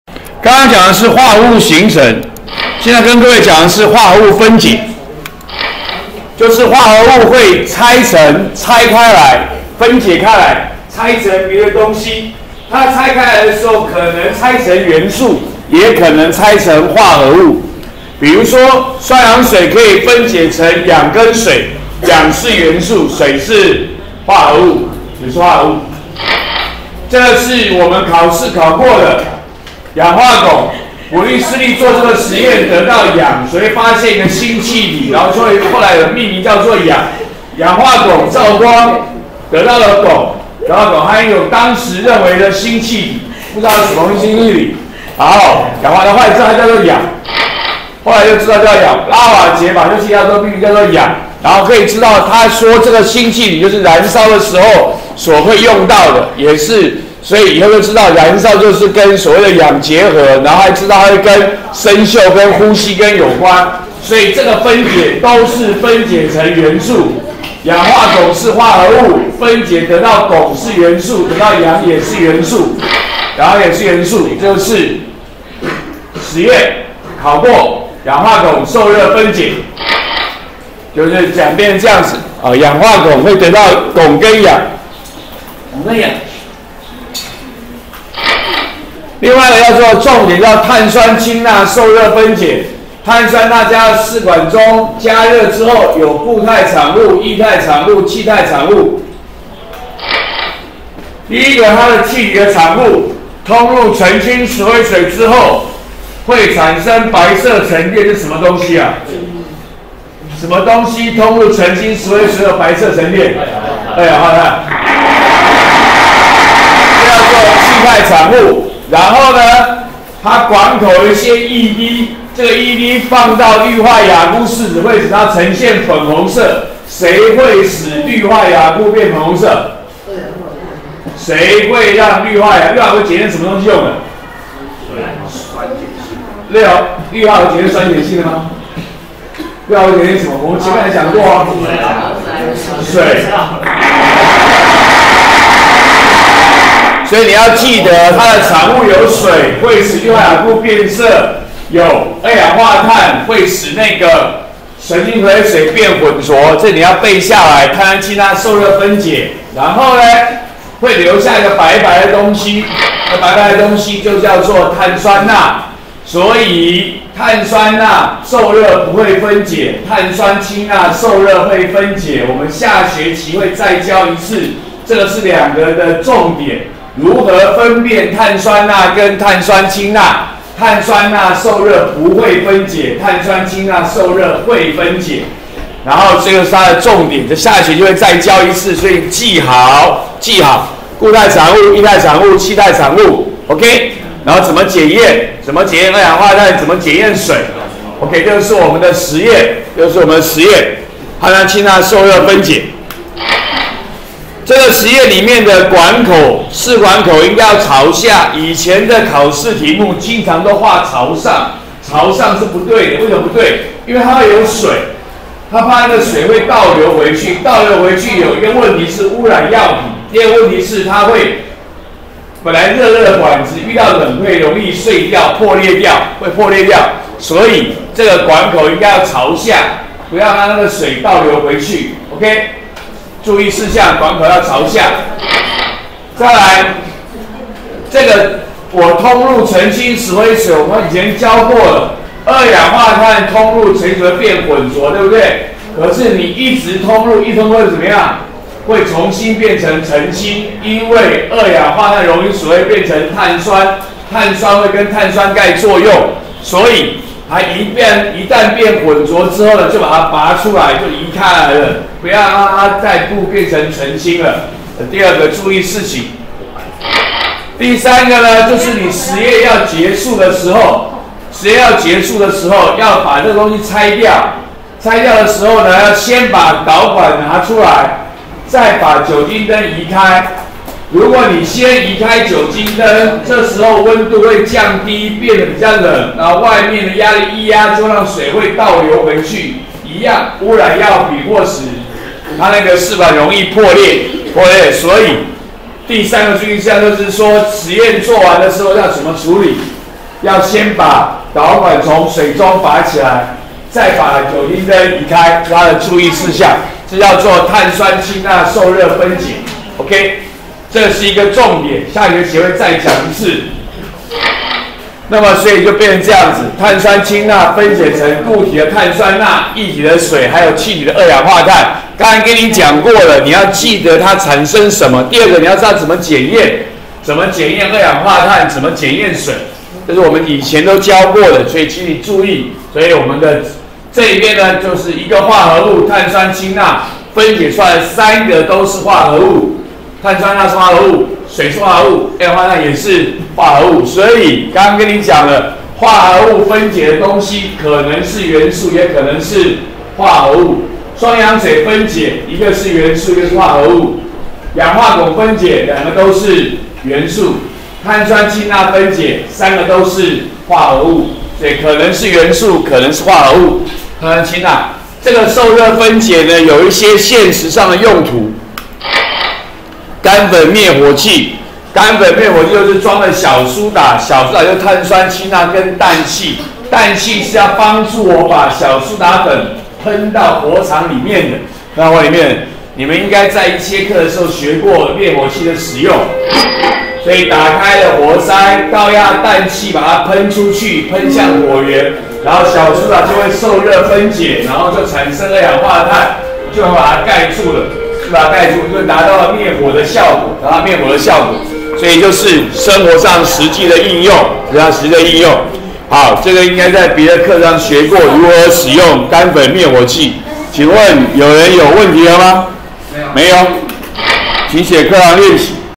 104學年國二理化＿元素與化合物05：化合物分解；氧化汞和碳酸氫鈉分解
介紹化合物分解，包括雙氧水、氧化汞和碳酸氫鈉分解。說明碳酸氫鈉分解的實驗注意事項，及在生活中的應用。